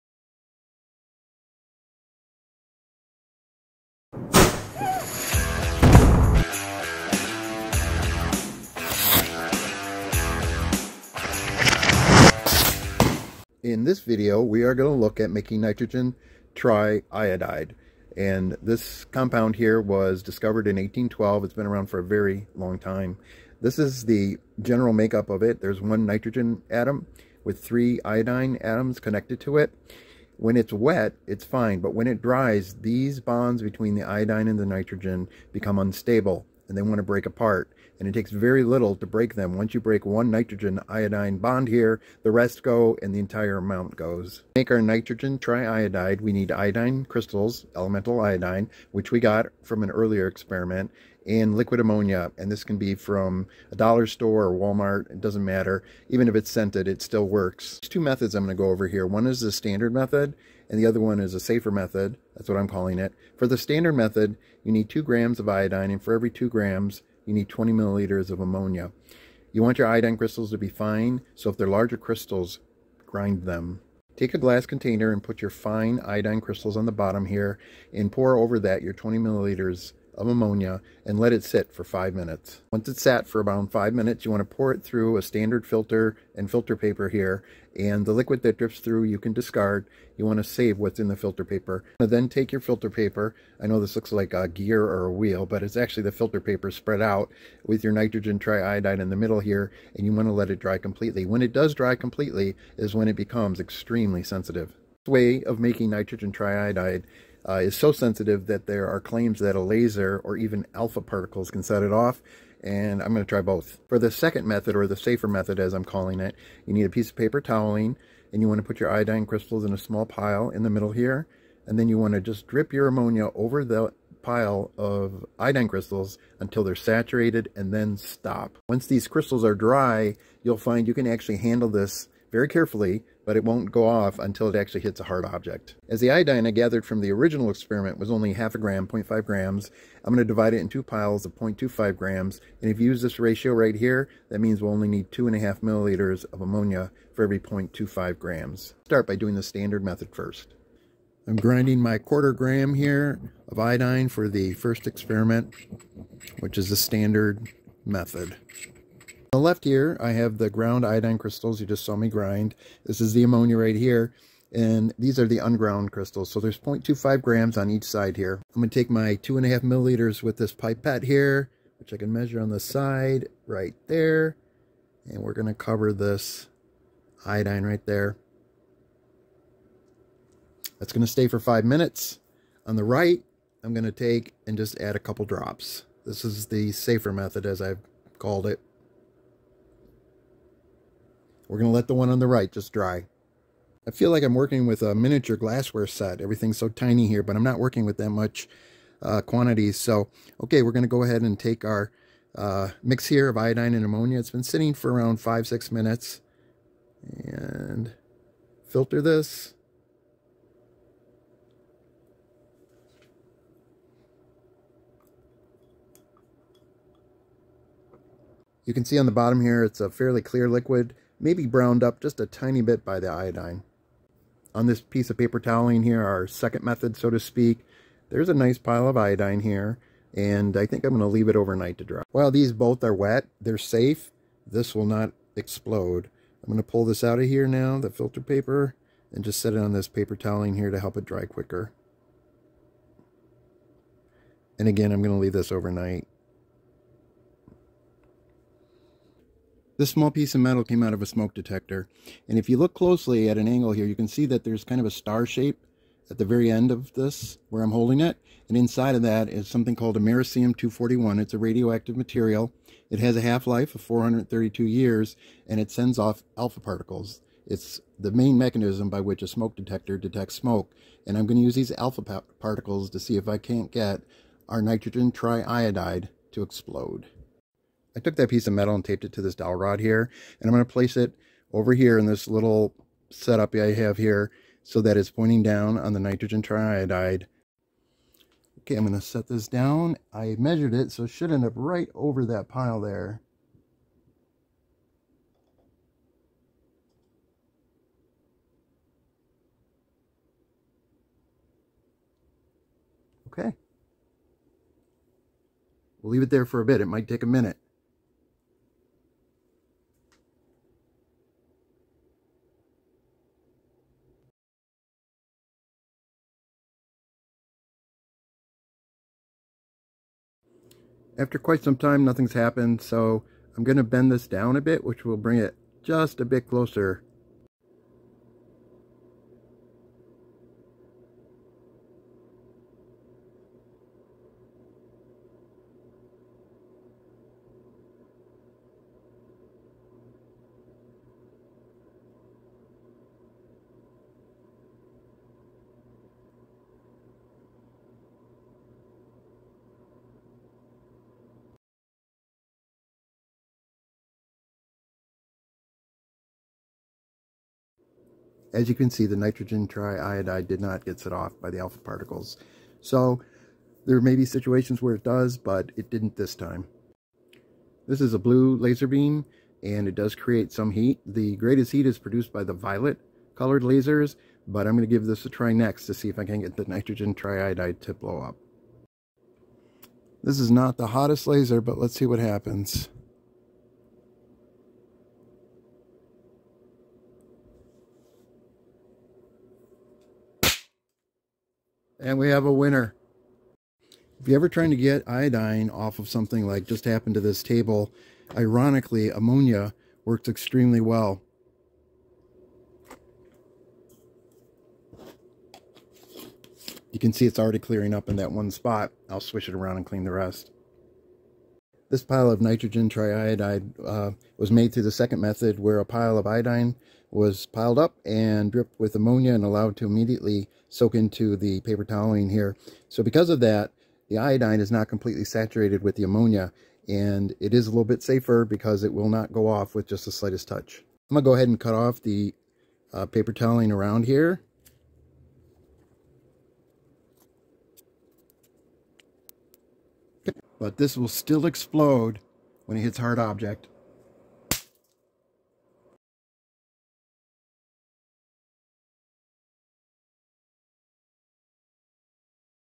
Making The Nitrogen Triiodide Explosive
This stuff is so sensitive that even a strong breeze can cause it to explode lightning fast.  Or, a laser.  It's sensitivity can be changed but only slightly.  This is for demonstration purposes only.  Please people, do not attempt.

Thank you, as even the smallest of donations help this channel!

This video is strictly for demonstration purposes only.